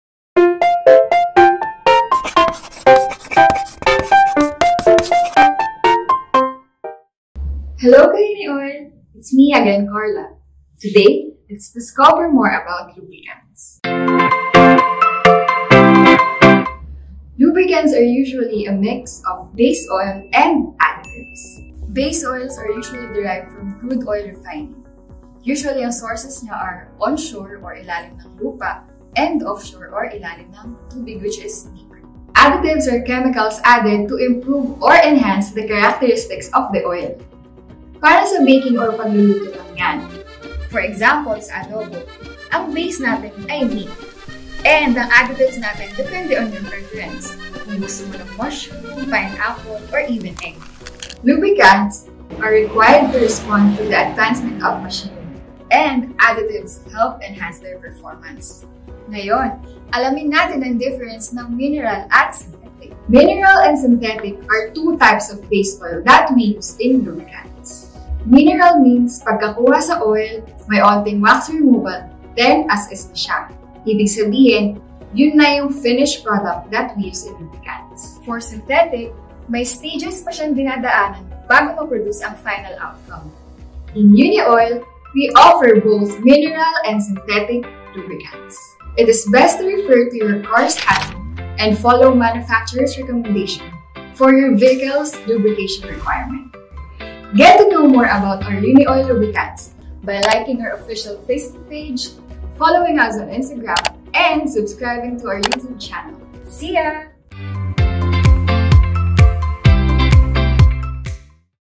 Know Your Lubes Episode 2: Mineral and Synthetic
Don’t know the difference of Mineral and synthetic?

Unioil presents: Know Your Lubes!

On this episode, find out what are the differences of mineral and synthetic lubricants.